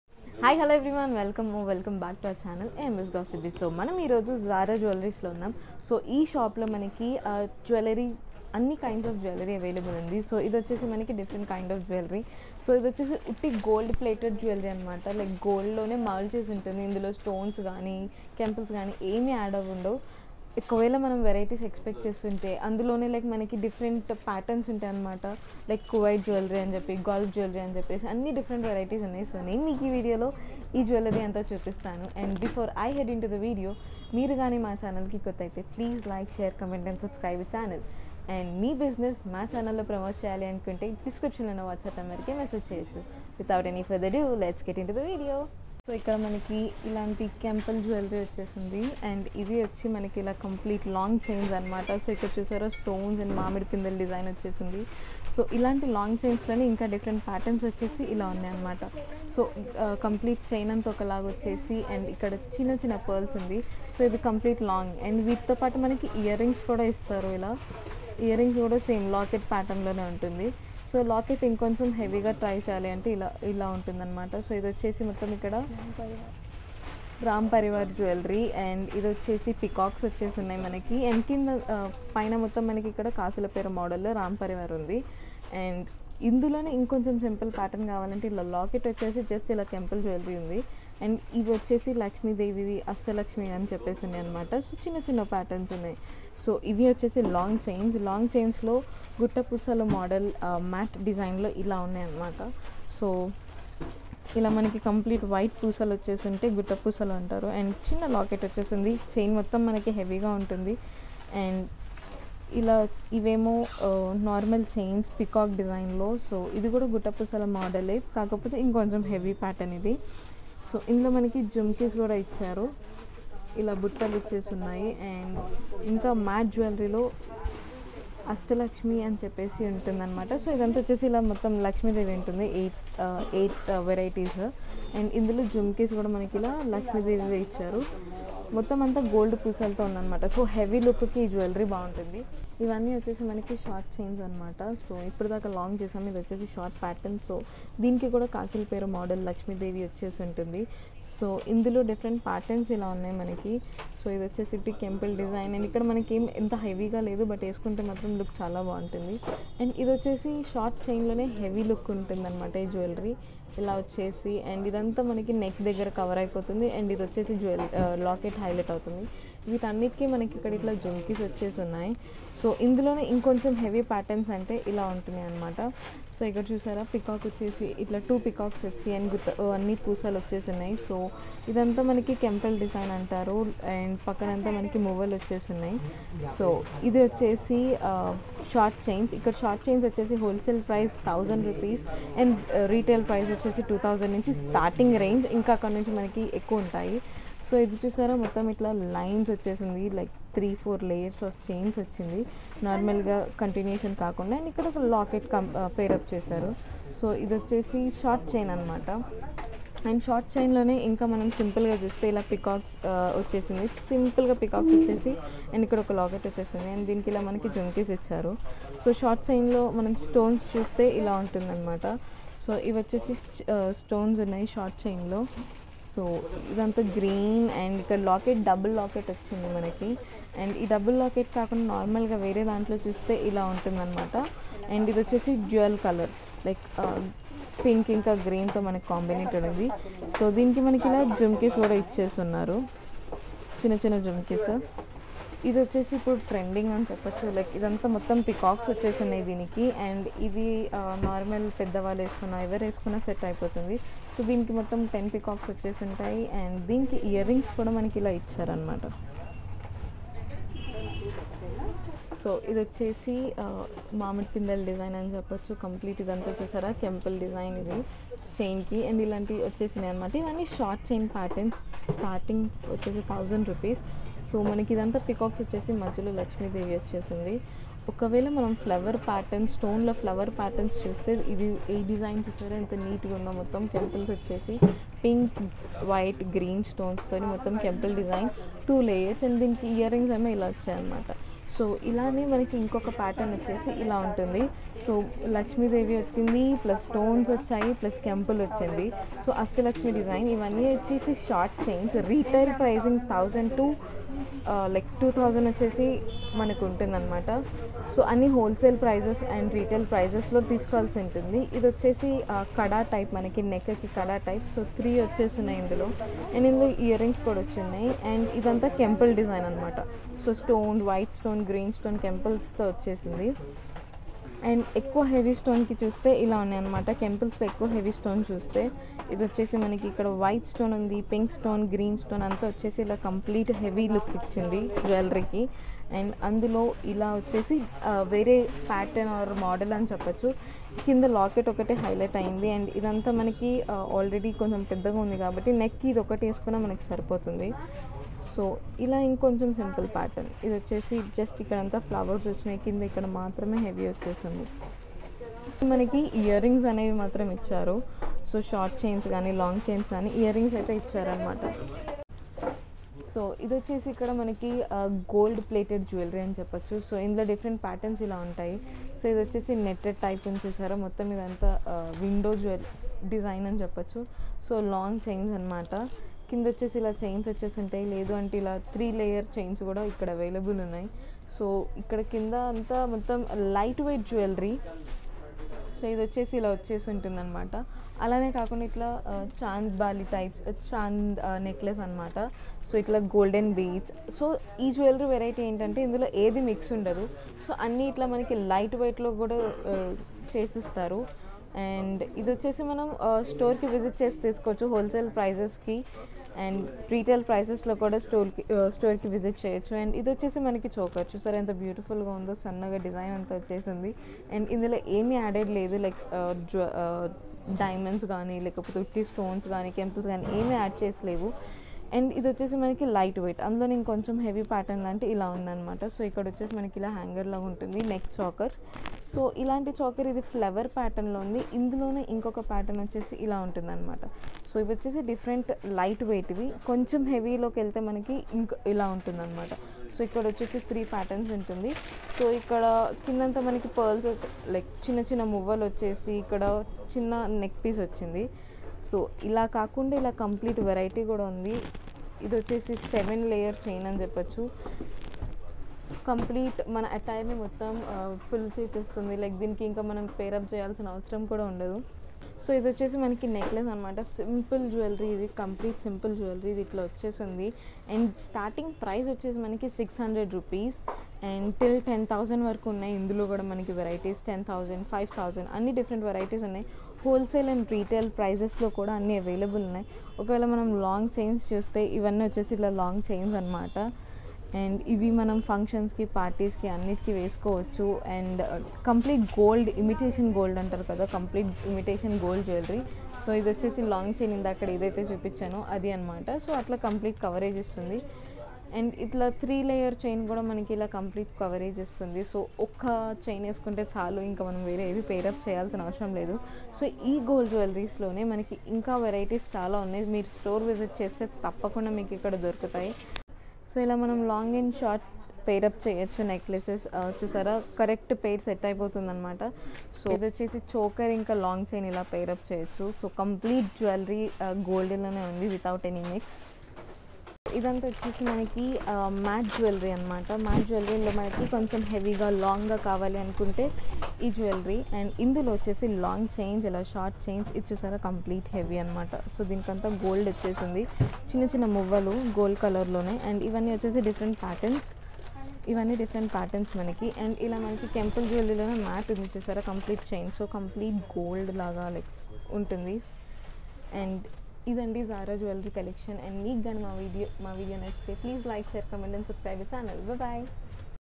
jewellery available in wholesale and retail | begum bazar |hyderabad |best prices |zara jewellery
shop name :- zara jewellery

Address: 15-6-45/3 opp osmania hospital, Begum bazar, Hyderabad

Have a good day,  Will meet you soon with next video

# jewellery
begum bazar jewellery manufacturers